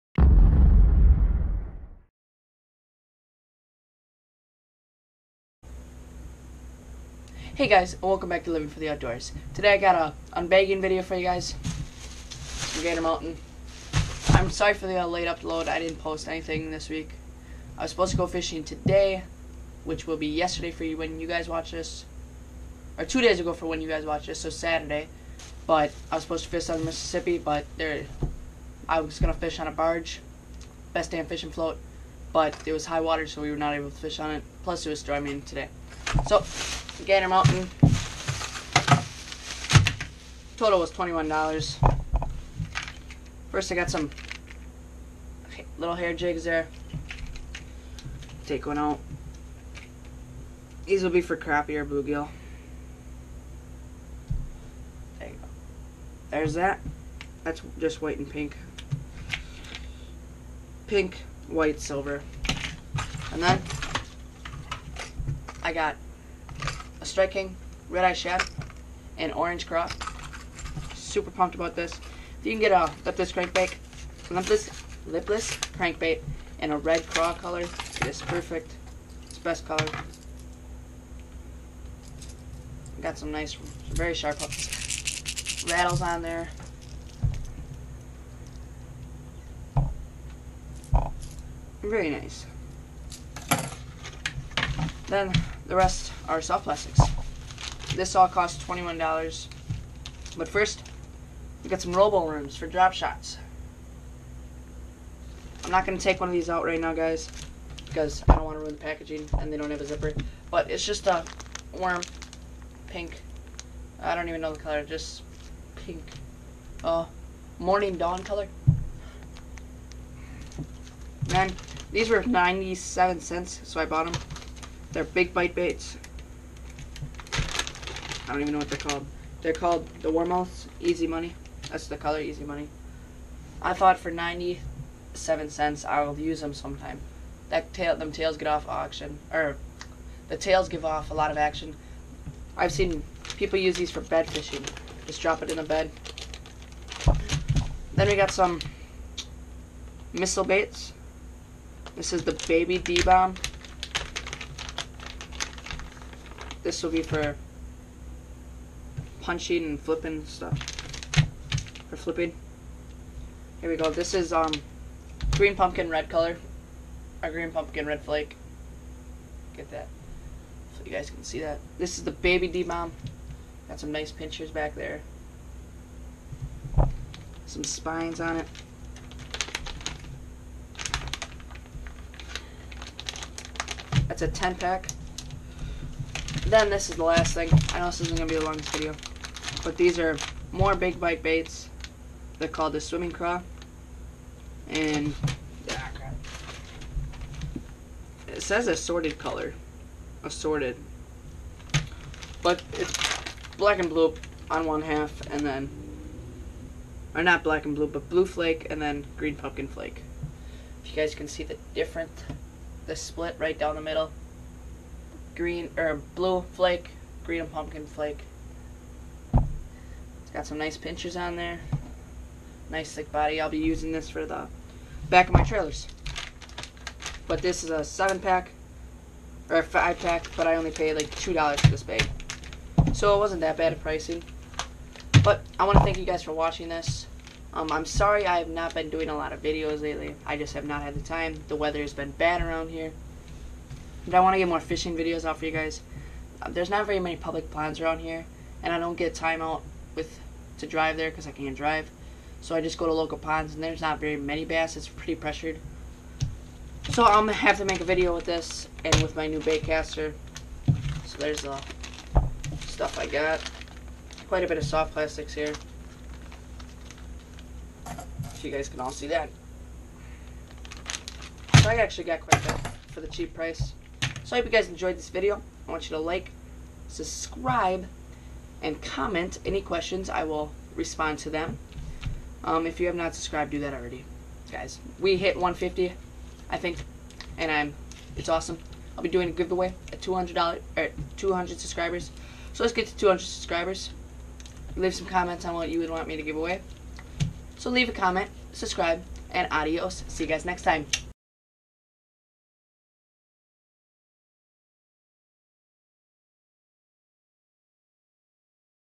Gander Mountain Unbagging $21
Gander mountain unbagging $21, here is my new video of me just getting some new products and showing you them. I hope you guys try some of these products. I am sorry that I have not been posting any fishing I am sorry on that. hope you enjoy